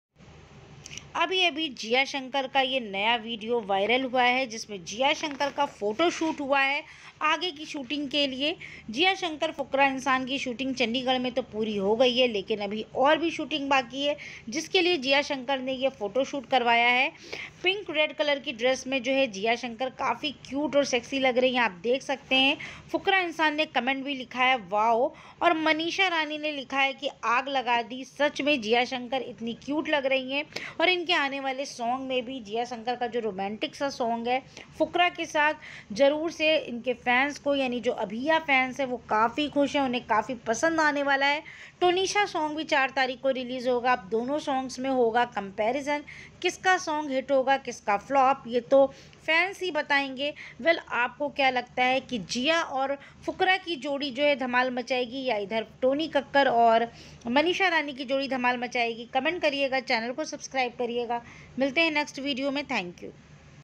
Jiya Shankar new photoshoot for new song with Fukra Insaan,Jiya looking so hot in pink, Elvish Yadav
Bigg Boss Ott 2 episode today

Bigg Boss OTT SEASON 2: Official Promo & Contestants List, Ranveer Singh, BB OTT 2 LIVE, Karan Johar